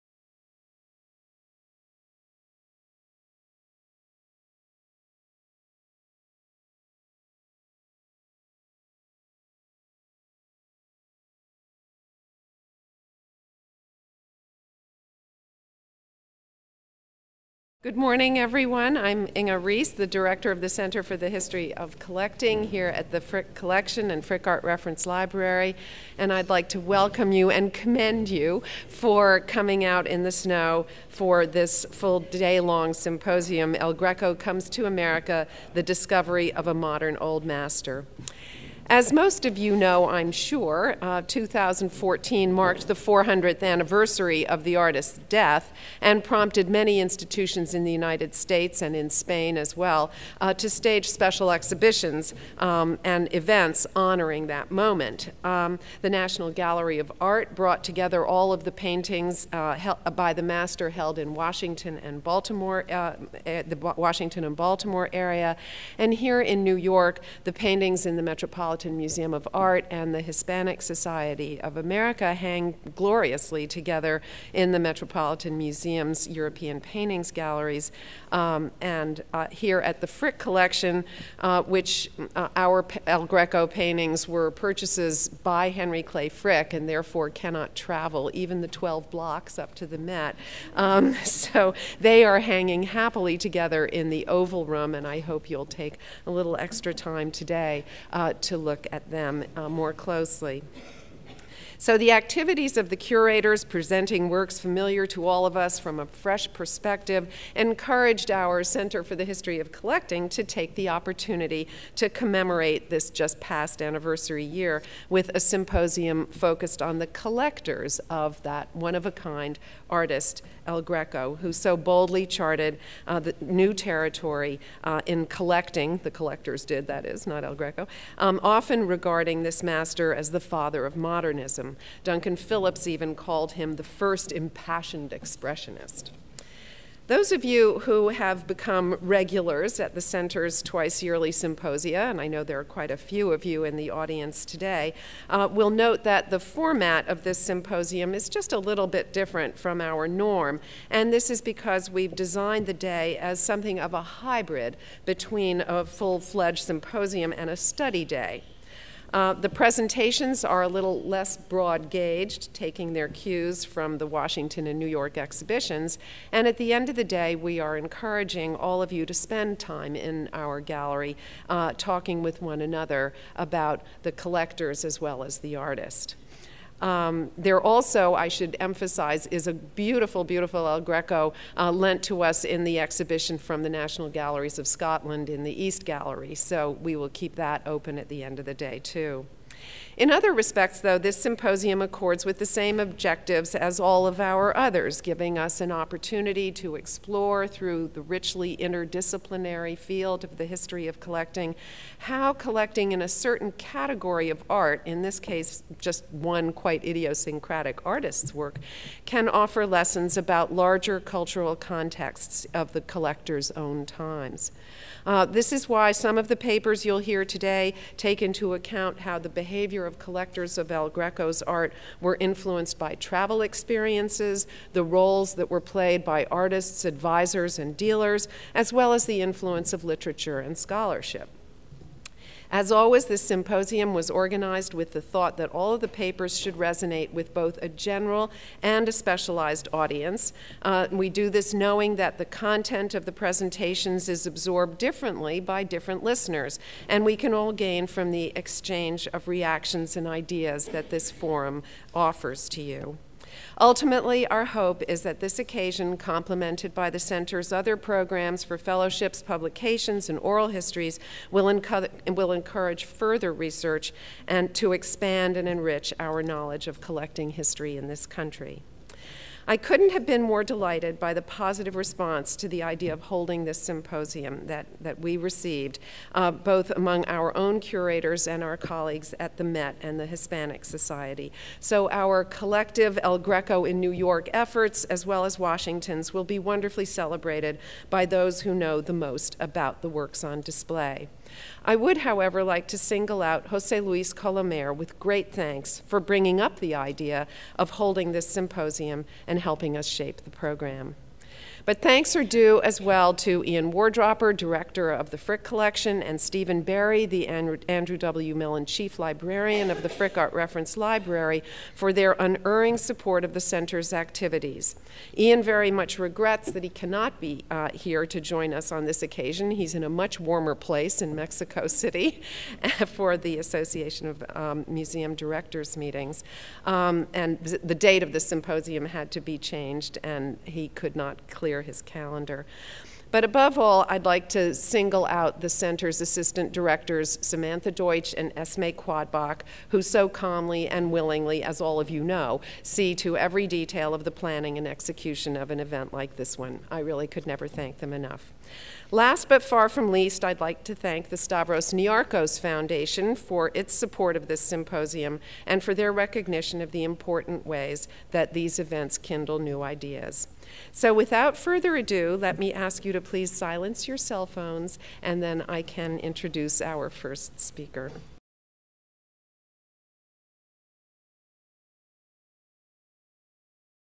Welcome to 'El Greco Comes to America: The Discovery of a Modern Old Master'
Inge Reist, Director of the Center for the History of Collecting at the Frick Art Reference Library, introduces a one-day symposium titled 'El Greco Comes to America: The Discovery of a Modern Old Master.' This event was organized by the Center for the History of Collecting at The Frick Collection.

Location: The Frick Collection, New York, NY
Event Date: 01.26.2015
Speaker: Inge Reist

[previously hosted on Vimeo: 192 views]